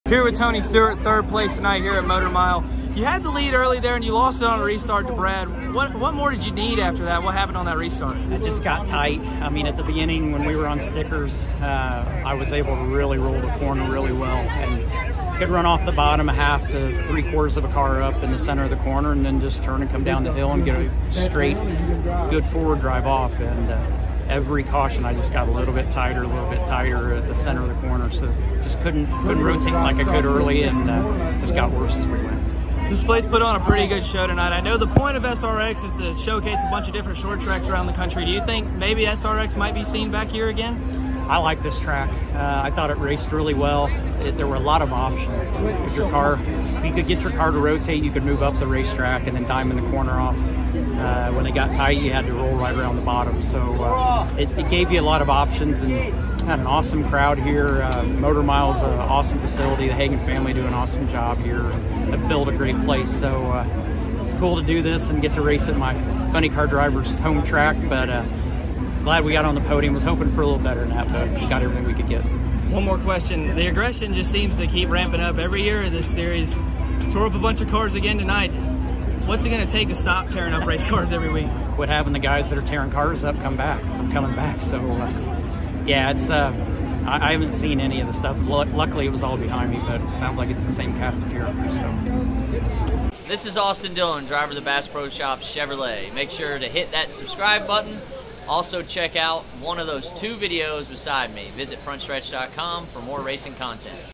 Tony Stewart On SRX Carnage: "Quit Having The Guys That Are Tearing Cars Up Come Back"
Frontstretch's Chase Folsom chats with Tony Stewart after Thursday's SRX race at Motor Mile